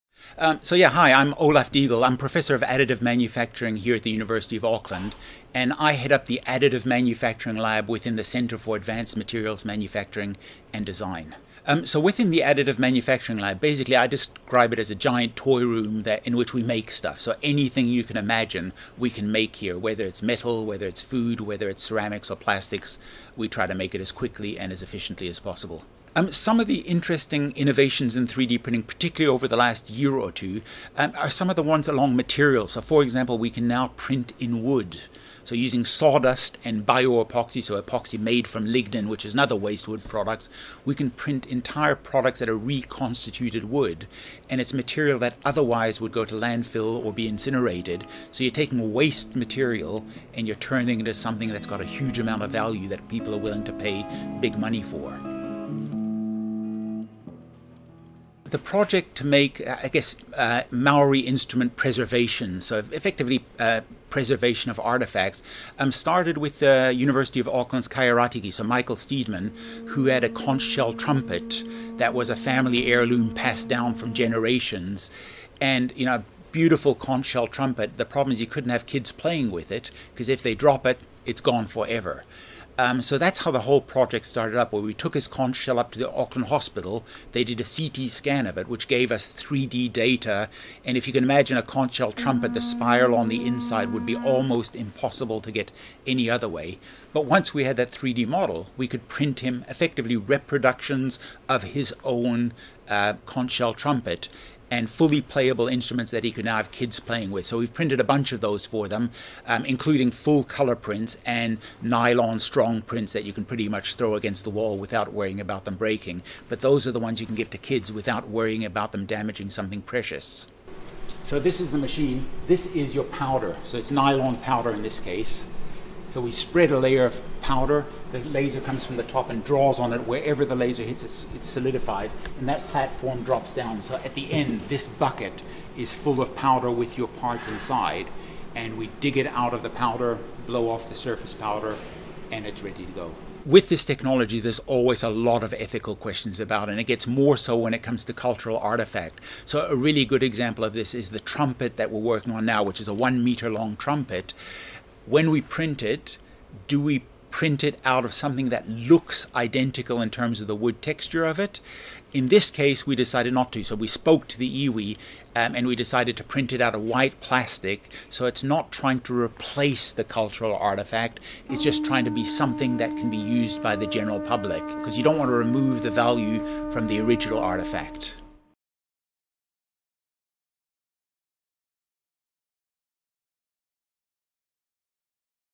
How 3D printing copies of taonga pūoro can protect the originals
Professor Olaf Diegel is using 3D printing to help preserve Aotearoa’s precious Māori musical instruments.